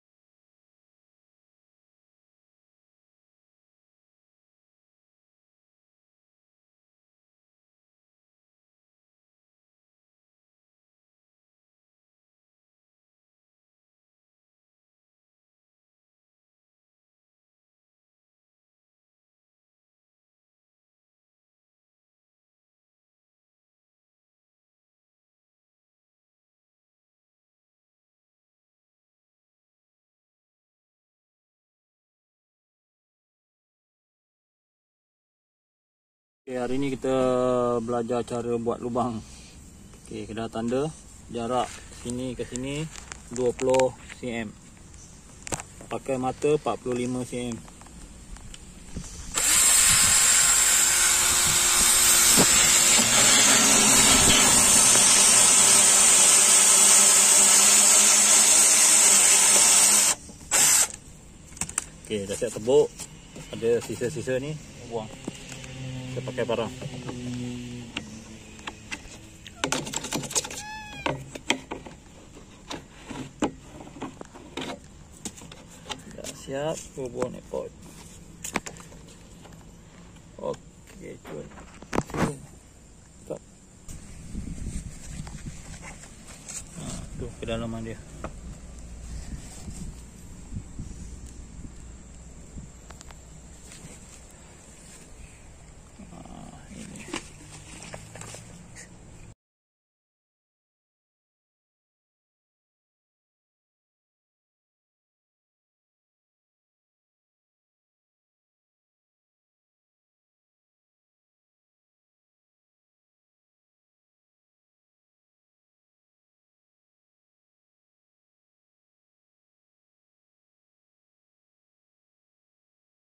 Hydroponik Takung Paip PVC
Cara Buat hydroponik takung menggunakan paip pvc bulat..

Dari Semaian hinggalah tuaian.

Medium menggunakan span.

Jika bermanfaat boleh share video ini.

Joint group Berkebun D Rumah untuk berkongsi suka Duka berkebun

Like n follow page Tukang Kebun utk info Tani.